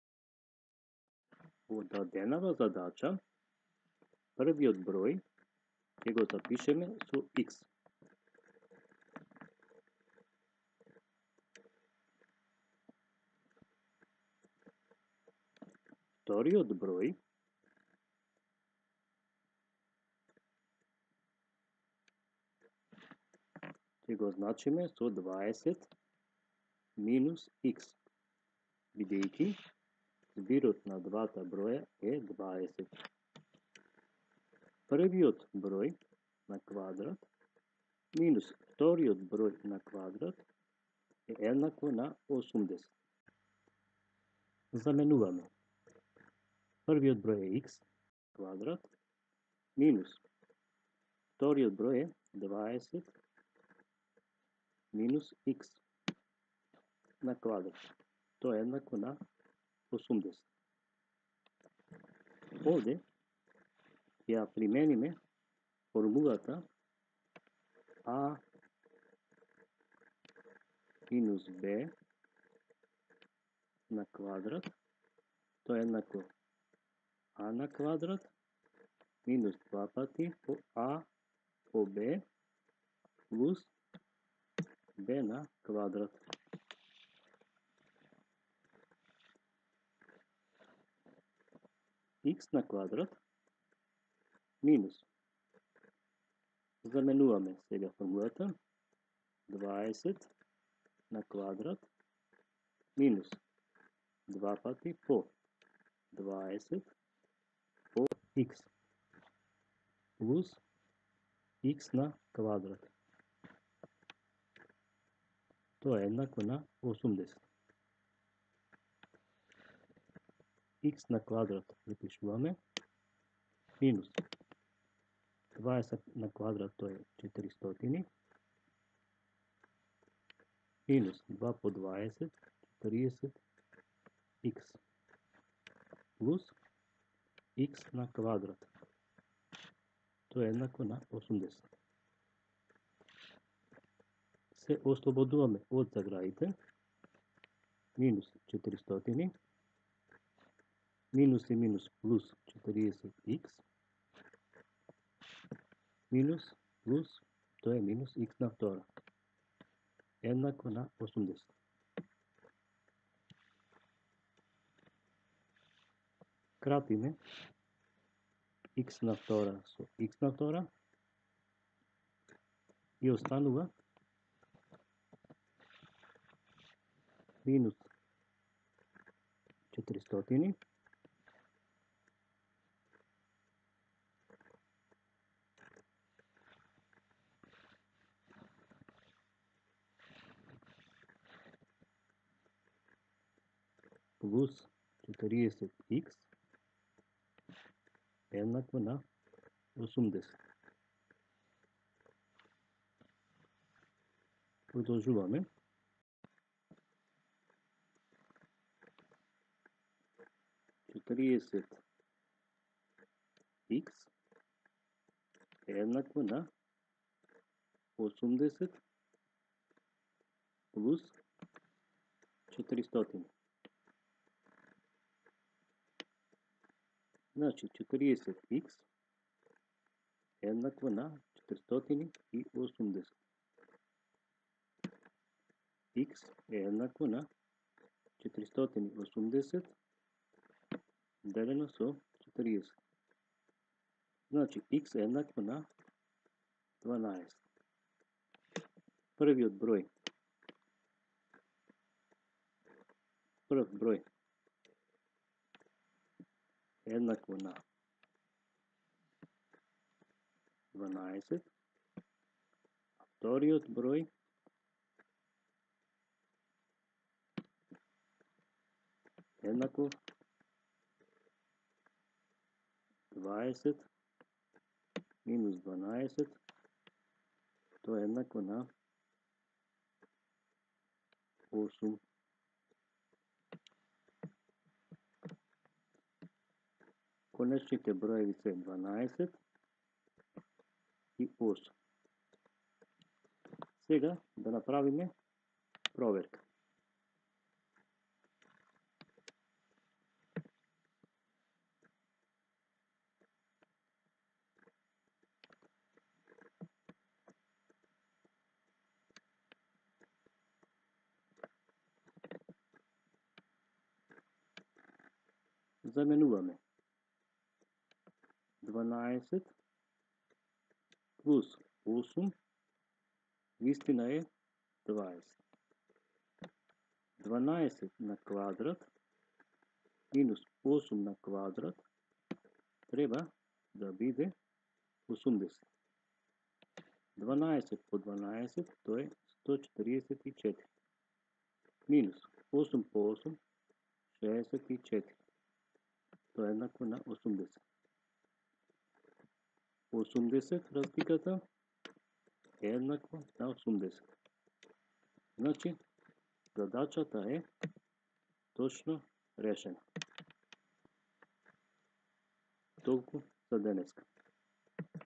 Primena na linearni ravenki
Primena na linearni ravenki  kaj prirodnite broevi
Se nadevam na podrška na sledniov kanal za edukacija